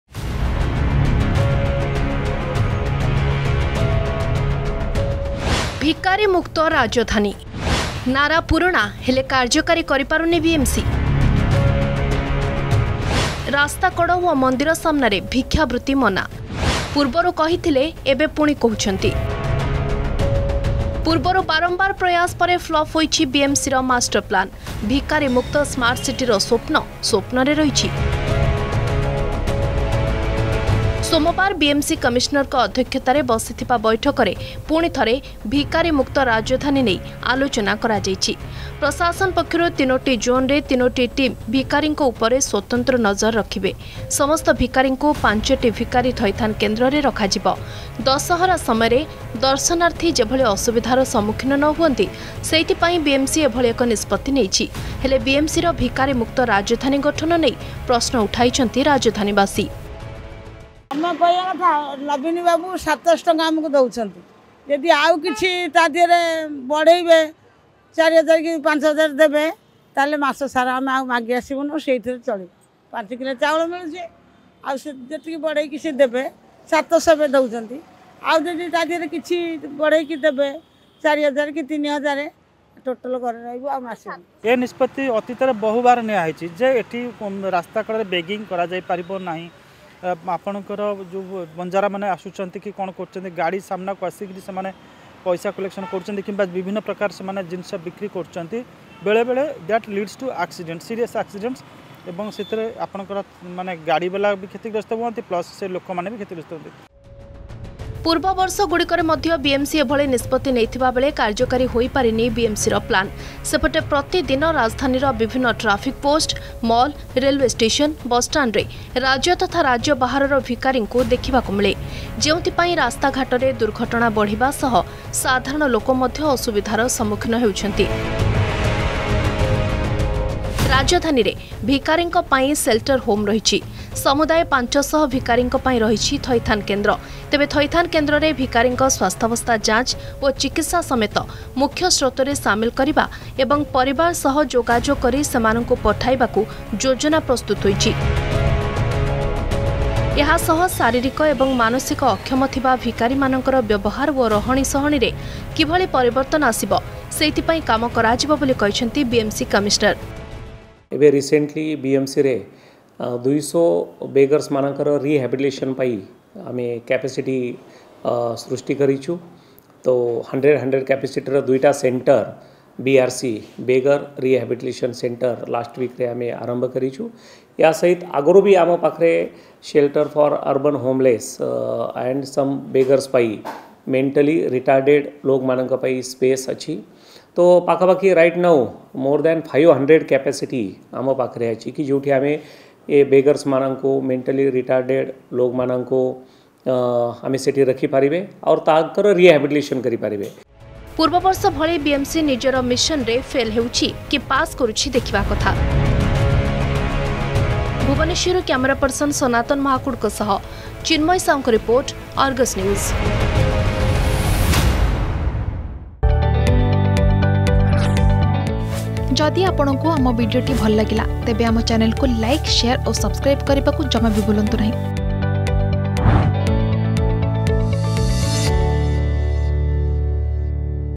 Bhubaneswar To Be Free Of Beggars | BMC
ପୁଣି ଭିକାରୀ ମୁକ୍ତ ରାଜଧାନୀ ମିଶନରେ ବିଏମସି । ରାସ୍ତାକଡ଼ ଓ ମନ୍ଦିର ସାମ୍ନାରେ ଭିକ୍ଷାବୃତ୍ତି ମନା । ନିୟମ ନମାନିଲେ ଉଠାଇନେବ ବିଏମସିର ସ୍ବତନ୍ତ୍ର ସ୍କ୍ବାର୍ଡ ।  ଭିକାରୀଙ୍କ ଉପରେ ନଜର ରଖିବା ପାଇଁ ୩ଟି ଜୋନରେ ୩ଟି ଟିମ ଗଠିତ । ସେପଟେ ଥଇଥାନକୁ ନେଇ ପ୍ରଶ୍ନ ଉଠାଇଲେ ରାଜଧାନୀ ବାସିନ୍ଦା ।